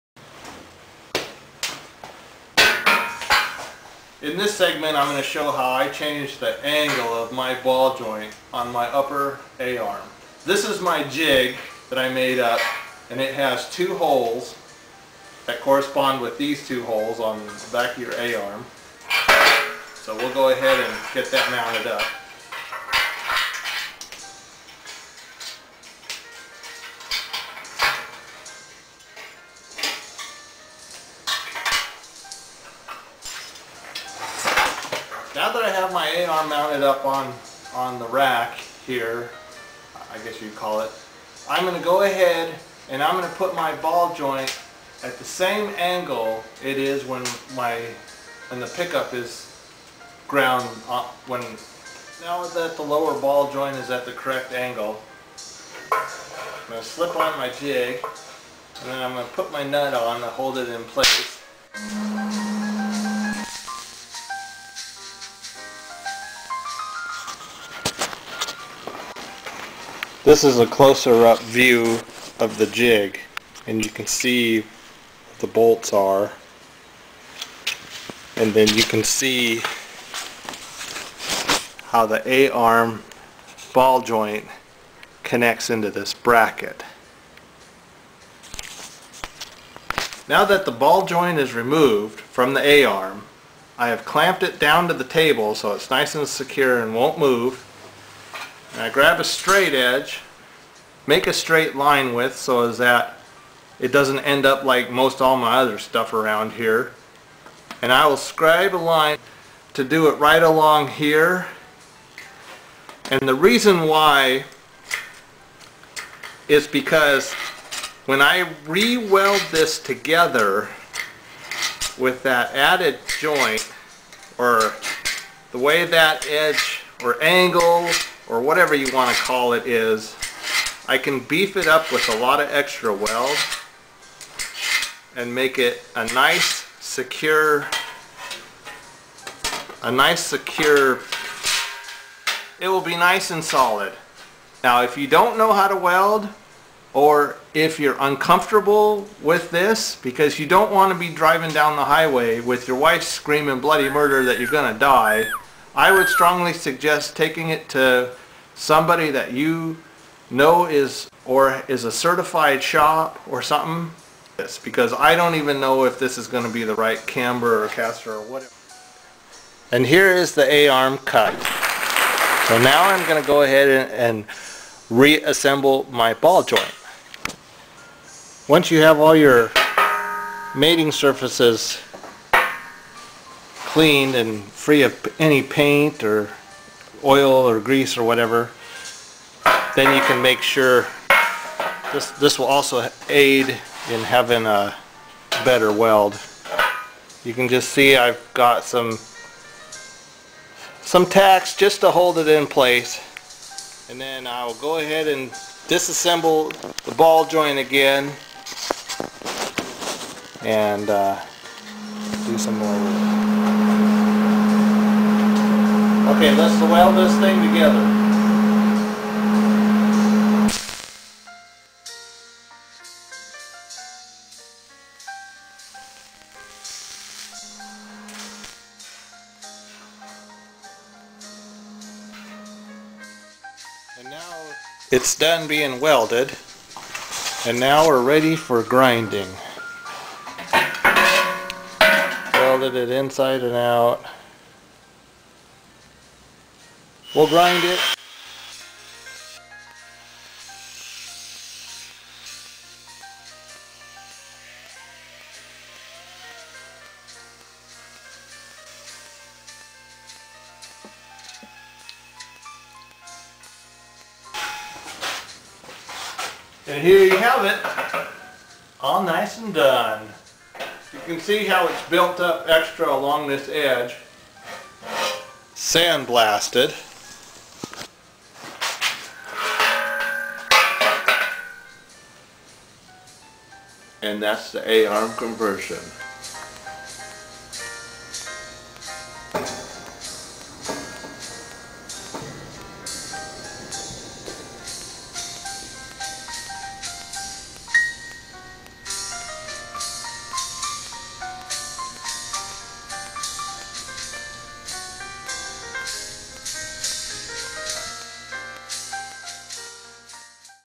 Bompula a arm rebuild
A brief vid on how I changed the ball joint a arm angle for the 1966 pick up